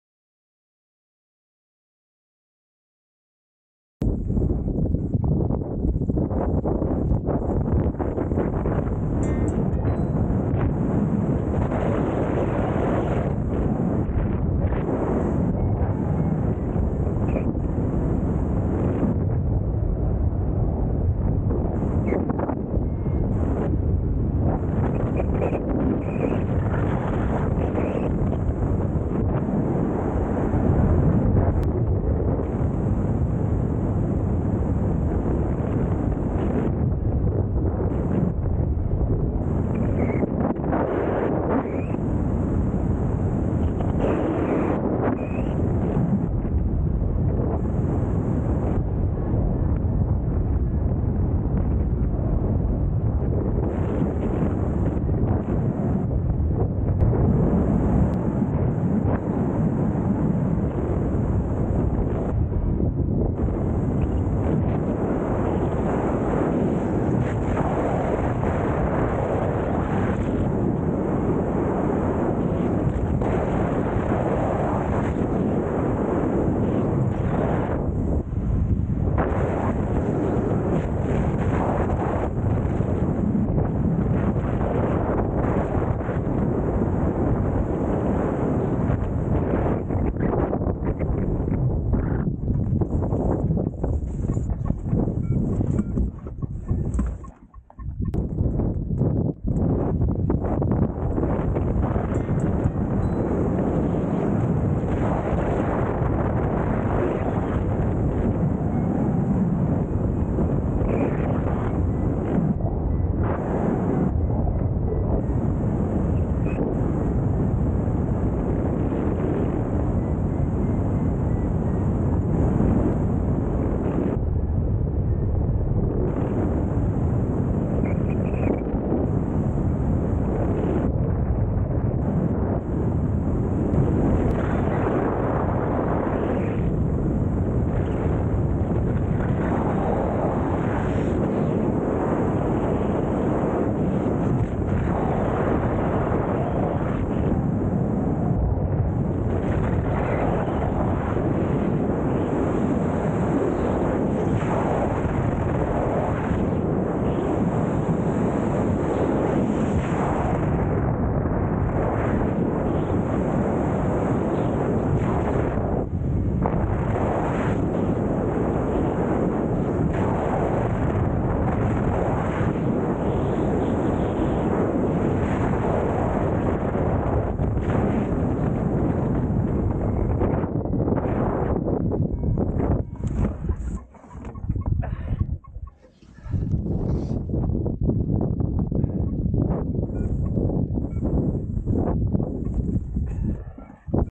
Tailbridge - 30th January 2024
Glider : Avian Puma
Wind : WNW, 20mph
Couple of flights in ridge lift. Both landings were not the best. First landing I made a quick decision which meant I had to turn over 180 degrees to get into wind (did this to avoid sinking air).  Second landing I struggled to turn on the downwind leg (turbulence) so went further back than planned.  That far back the wind gradient was massive which caught me out in the last few feet above the ground resulting in a belly flop.  No harm done in either landing though.